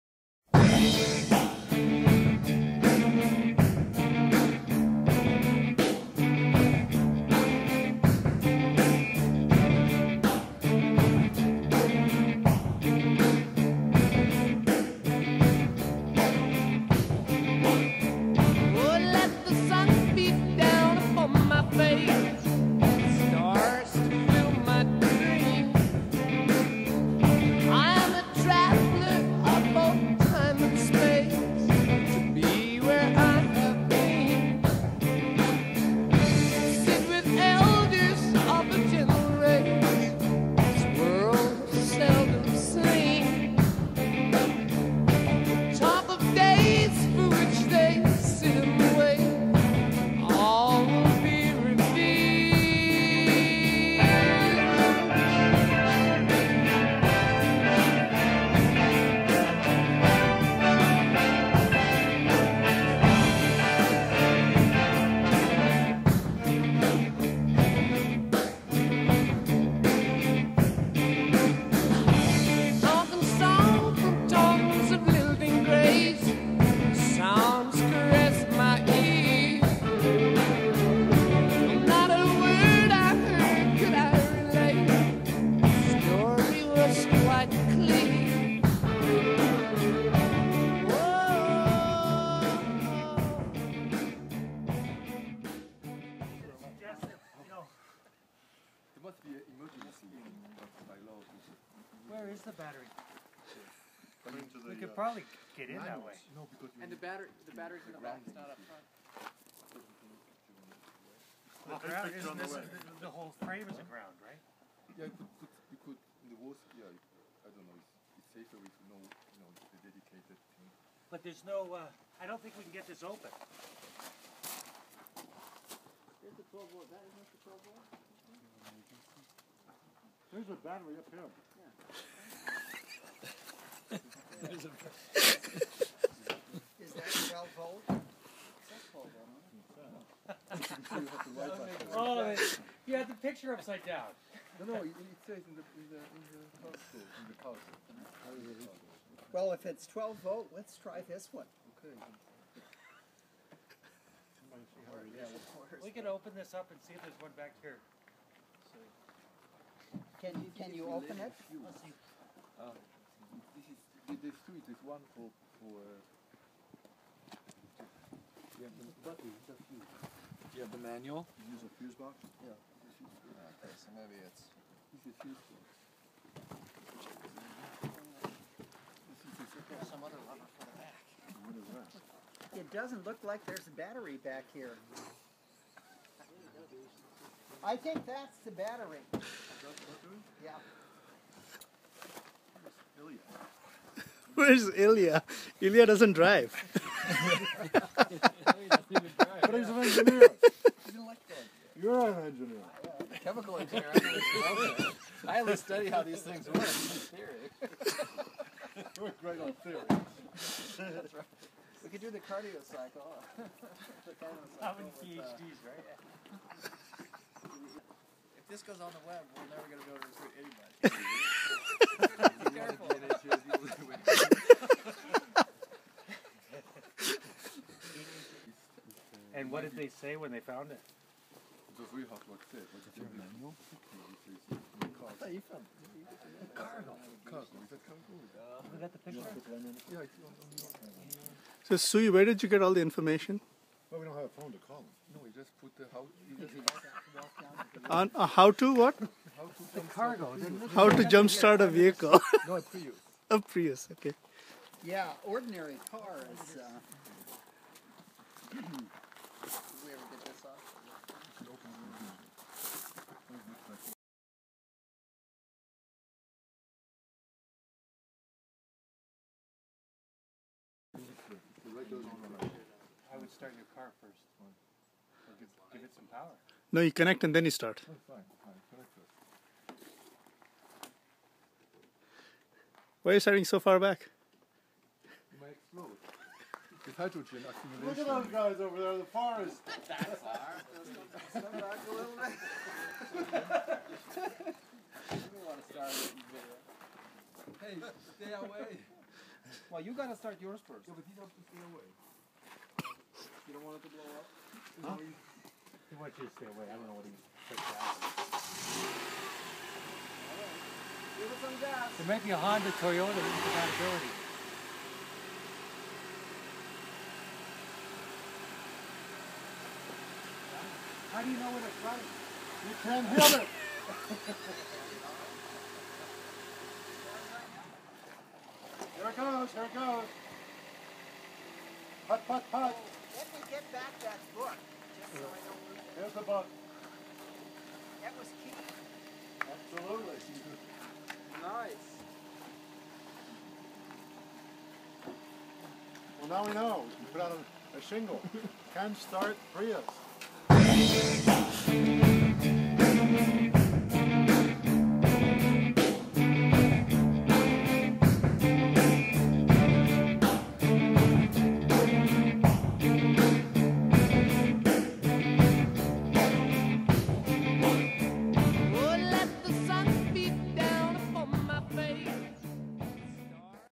Lee Hood and a Systems Biology Approach to Finding a Prius Battery
On Oct. 9 as ISB faculty were preparing to depart after an offsite retreat, Lee Hood discovered his Prius was dead. How many PhDs does it take to jump start a Prius? Watch as the brilliant minds who pioneered systems biology use cross-disciplinary collaboration to start Lee Hood's car.